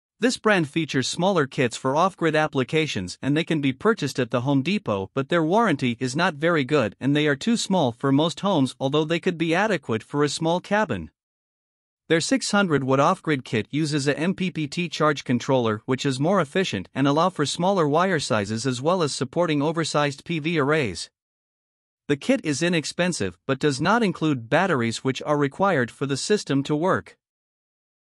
HvacRepairGuy 2023 Grape Brand Residential Solar System Reviews
In this video, HvacRepairGuy reviews the 2023 Grape brand residential solar kits. These reviews are for the 600 watt off-grid kits.

You can get the reviews of all of the brands where you can also compare the brands side by side using our Head To Head comparison tool.